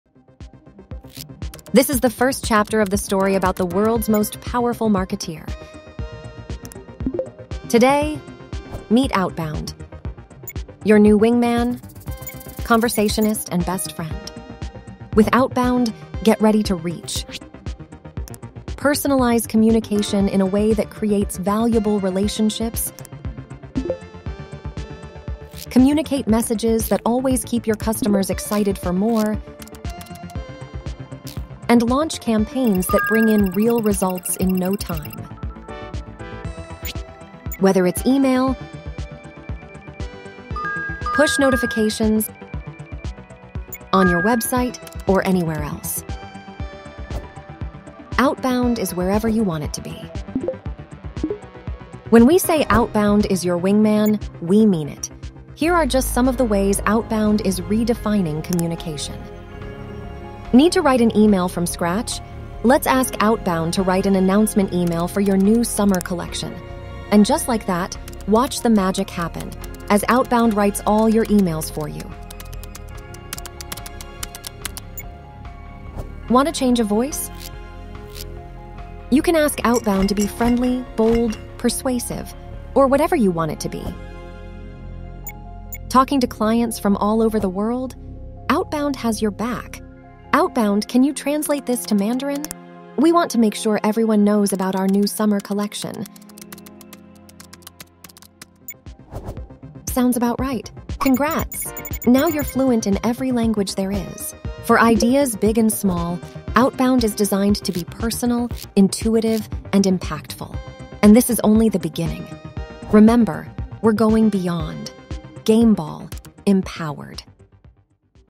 Gameball - Empowered: Meet Outbound
Outbound is Gameball's latest feature release designed to facilitate and automate communication, allowing you to send personalized communication to your customers through various channels, such as emails, mobile notifications, and web messages. With this feature, customers can send branding and promotional communication to all or specific segments of their base and quantify the impact of each campaign by setting goals.